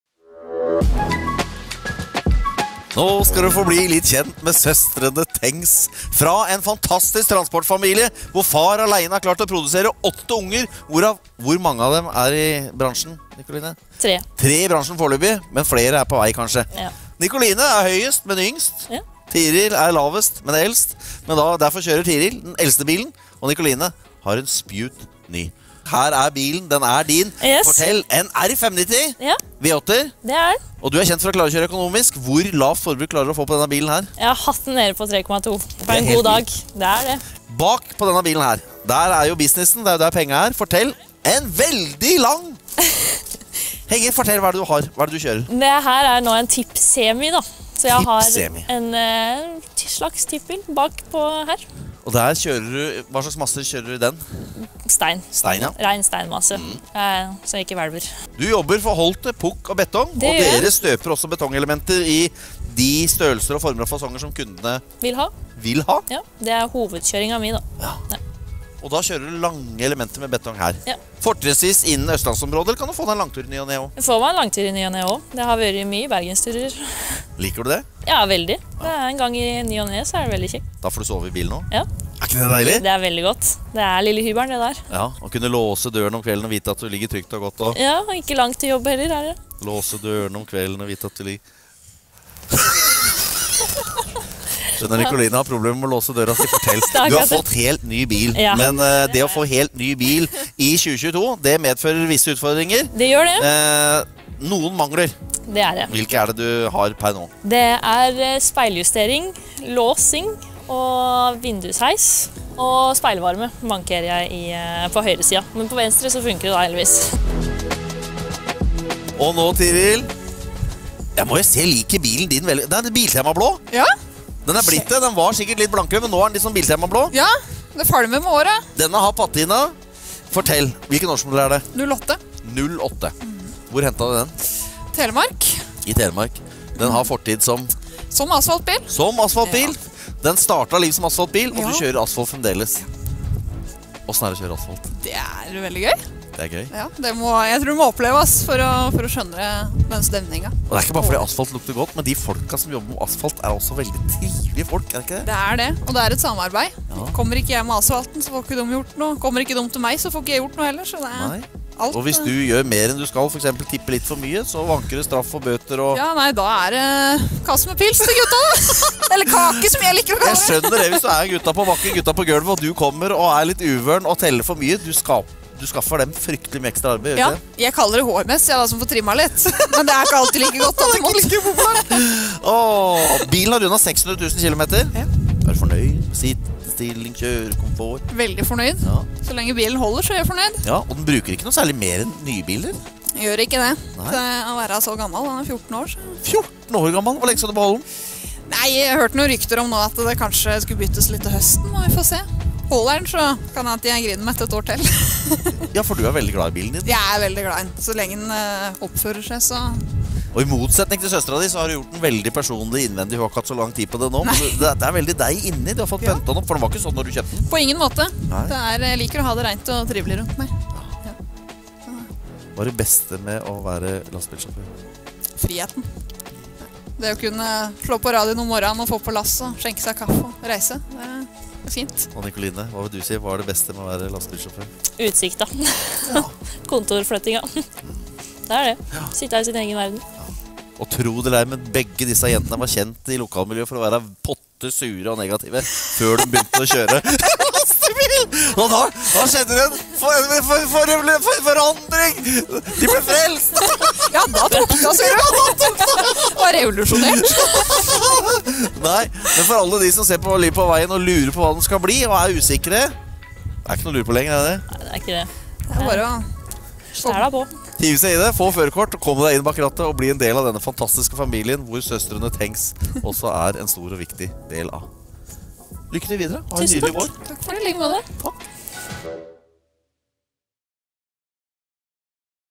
Søstre med lastebil
Ute på veien møtte vi disse herlige jentene, Tiril og Nikoline Thengs som kommer fra en real lastebilfamilie!